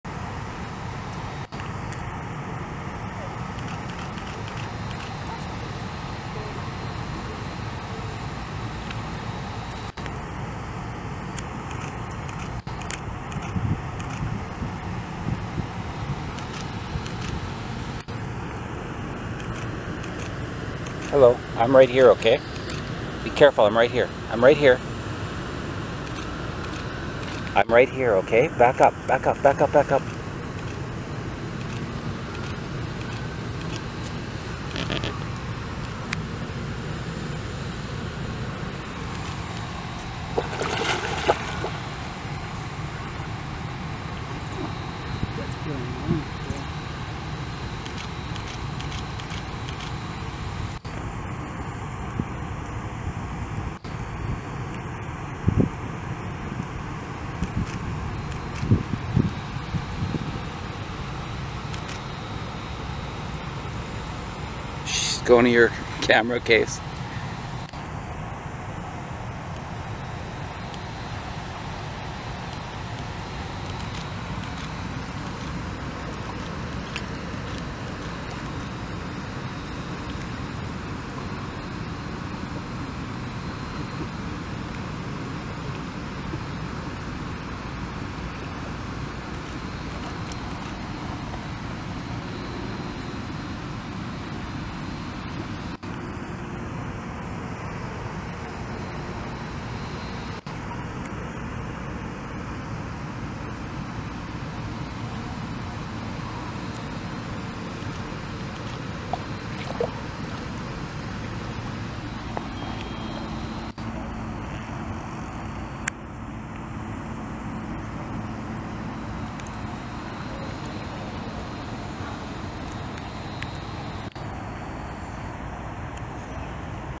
Amazing Up Close Visit with 4 Wild Beavers ...Curious Momma Beaver Smells my Shoe and a Camera Case
Tonight we had an amazing up close encounter with this family of 4 beavers.  Momma Beaver kept coming even as I tried to warn her I was there. There was so much beaver activity going on I did not know which way to point my camera.  Filmed in Saskatoon, Saskatchewan on July 21, 2021.